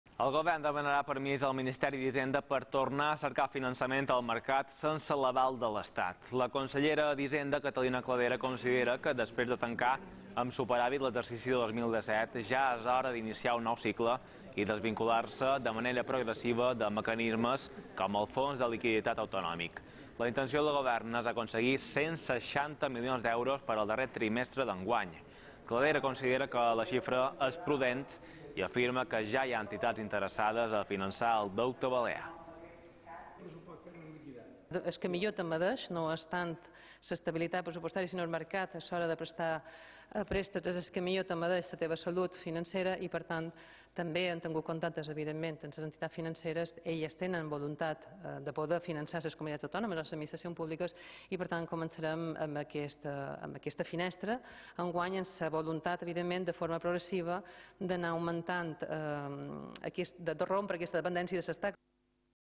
Les Balears vol tornar al mercat del deute al marge de l’Estat
El Govern demanarà permís al Ministeri d’Hisenda per tornar a cercar finançament al mercat sense l’aval de l’Estat. La consellera d’Hisenda, Catalina Cladera, considera que, després de tancar amb superàvit l’exercici de 2017, ja és hora d’iniciar un nou cicle i desvincular-se de manera progressiva de mecanismes com el Fons de Liquiditat Autonòmic. La intenció del Govern és aconseguir 160 milions d’euros per al darrer trimestre d’enguany. Cladera considera que la xifra és prudent, i afirma que ja hi ha entitats interessades a finançar el deute balear.